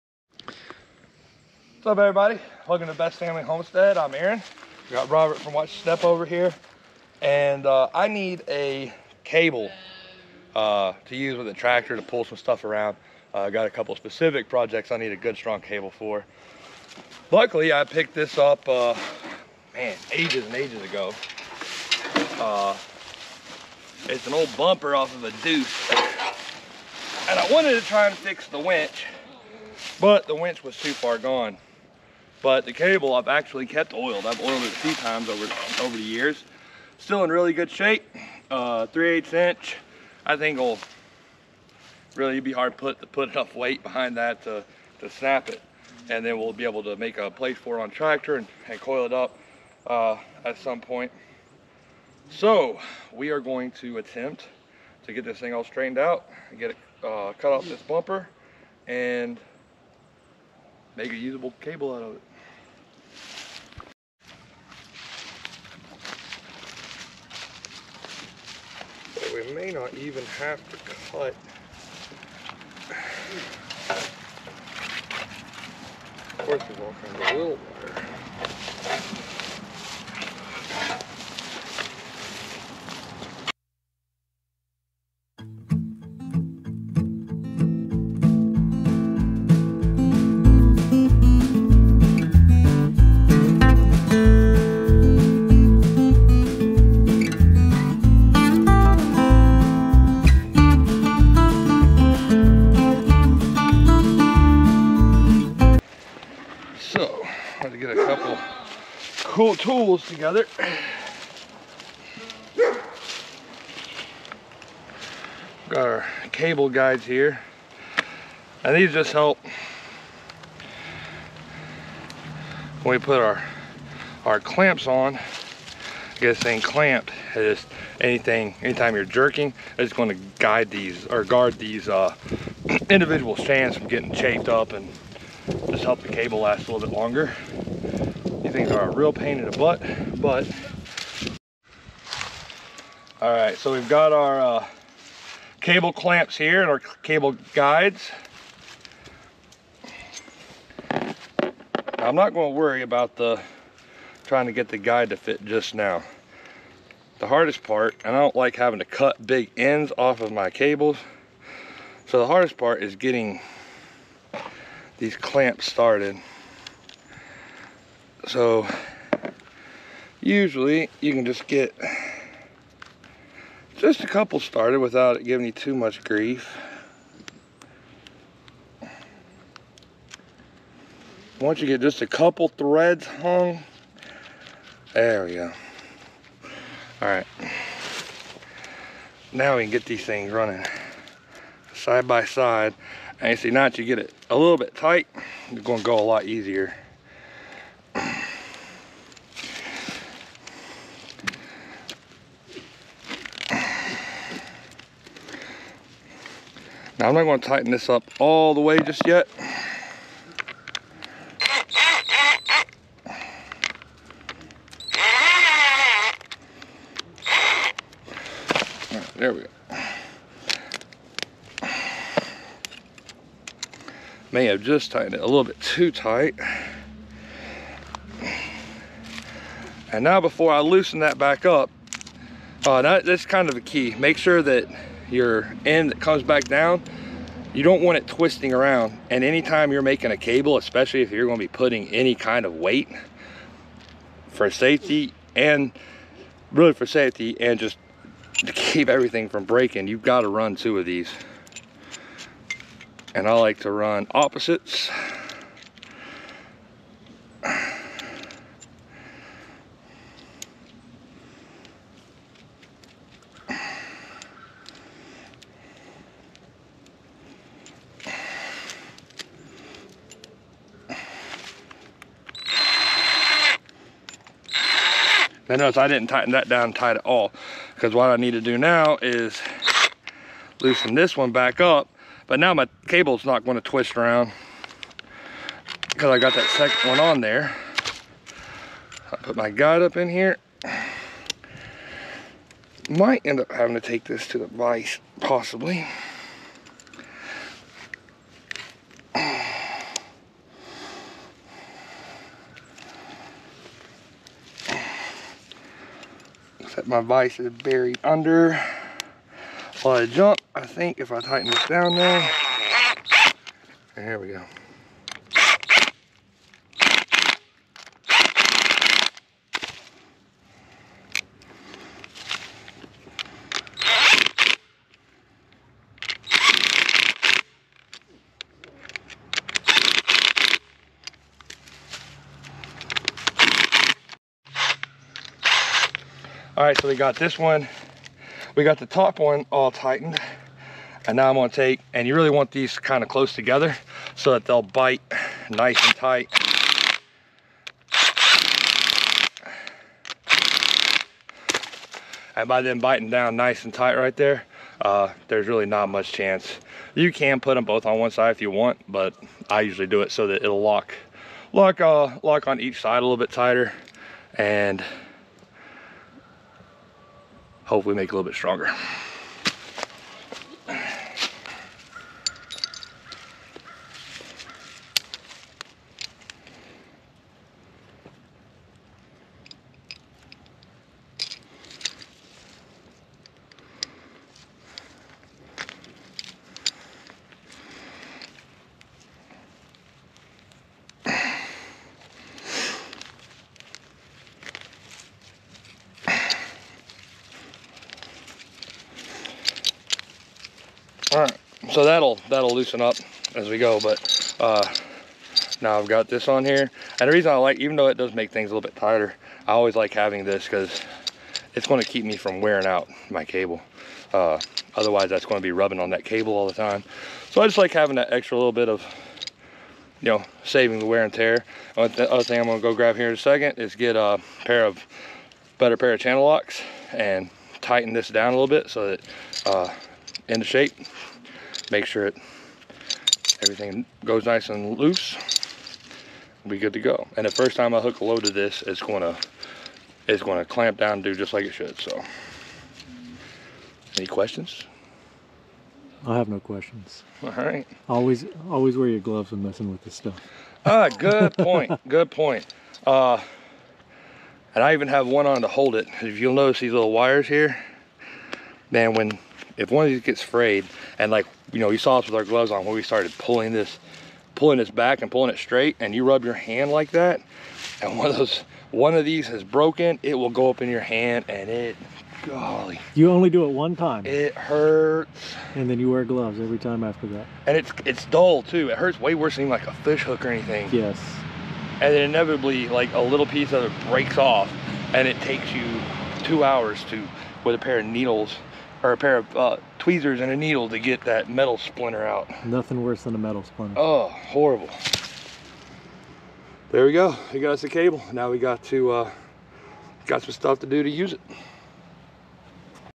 REPURPOSING OLD MILITARY WINCH CABLE ON 194 YEAR OLD HOMESTEAD | HOW TO RIG A CABLE TOW CABLE
I reuse a salvaged cable from an old Army duece to use as a tow cable around the homestead.